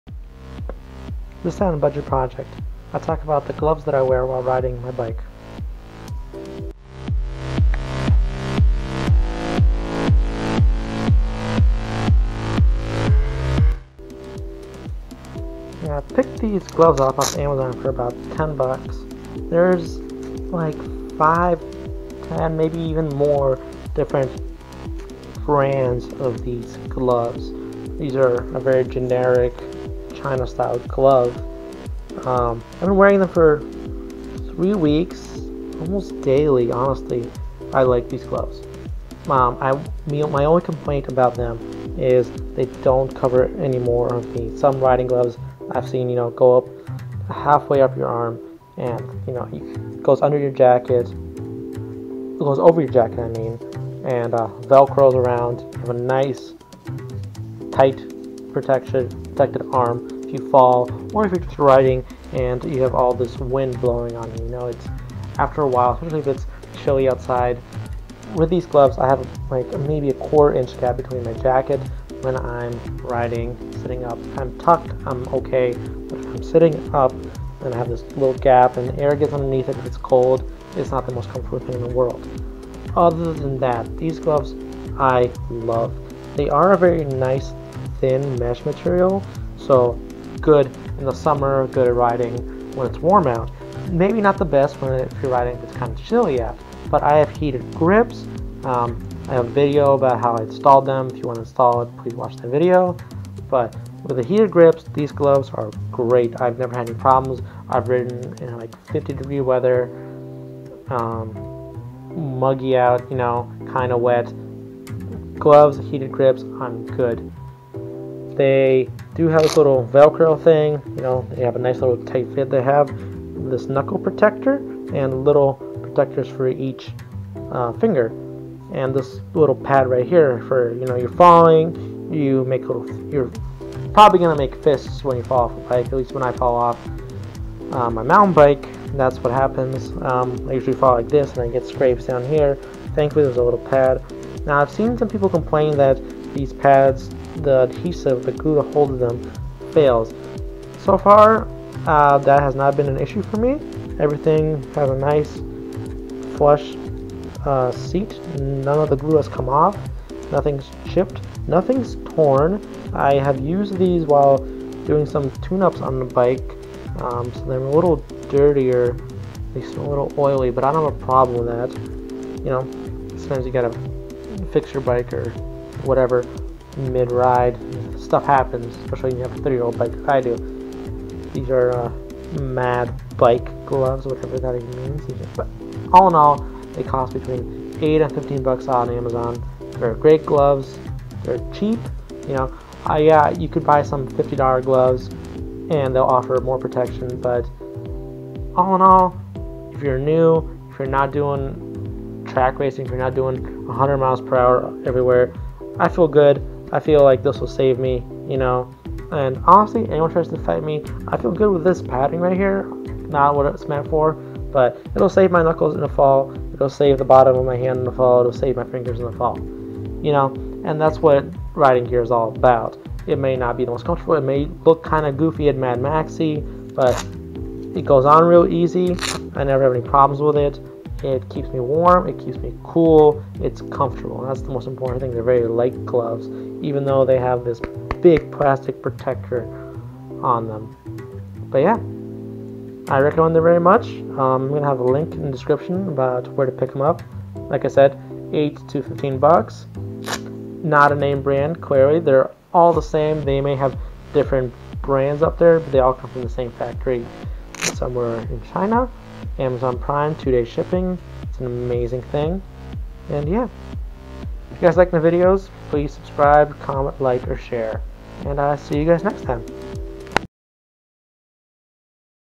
Riding Gear - $9 Riding Gloves
On this episode of Budget Project, I review the $9 riding gloves I wear while riding.

Filmed with the Geekpro 4